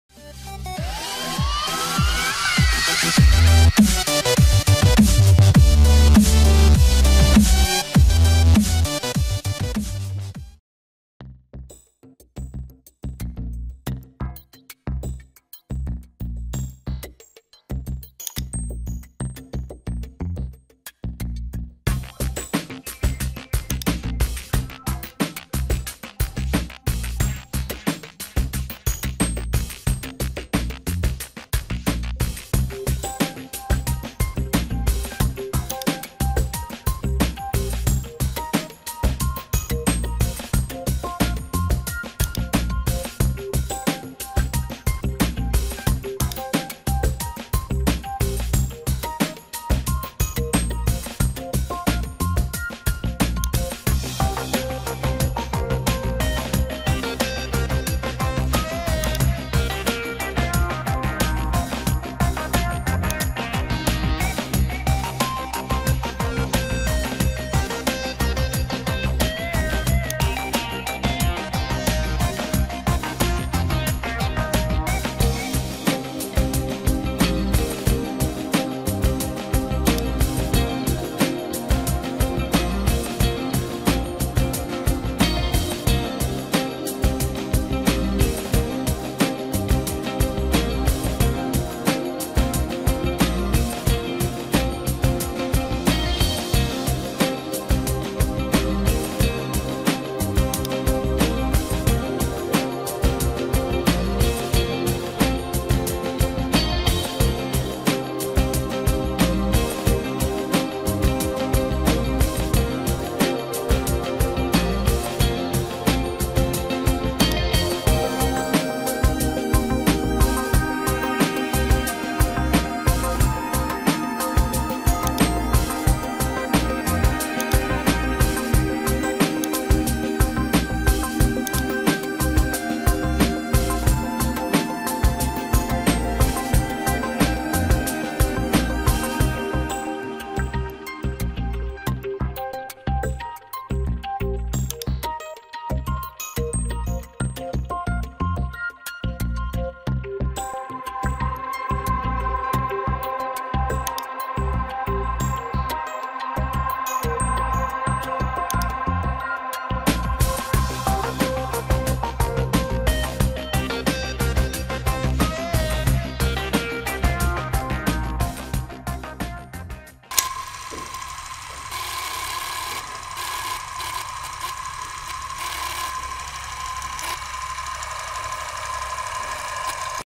How to Access a Web Page When It’s Down
In this video tutorial, I'll be showing you how to access a web page when It’s down.